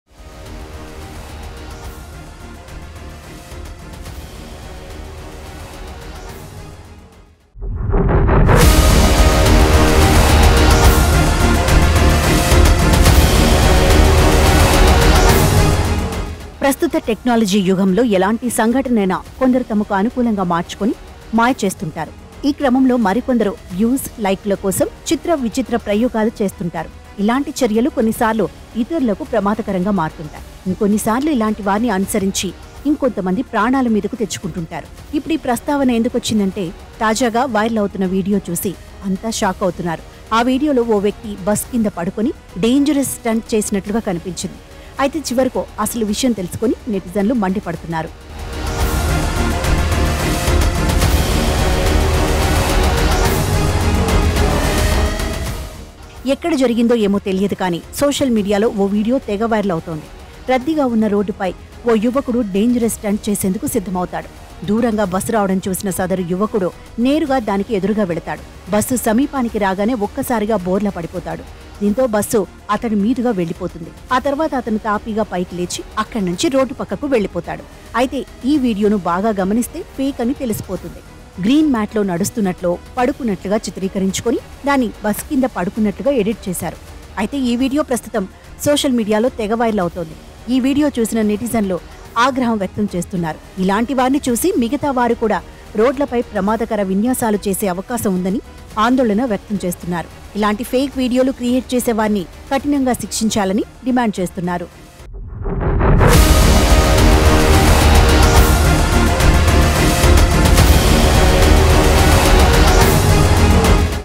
Under Running Bus :ఓరి నీ ధైర్యం పాడుగాను.. బస్సు కింద పడి ఇదేం పని సామి..! | ABN Digital Exclusives
Viral 'stunt' of man lying under a bus in Hyderabad is sly VFX trickery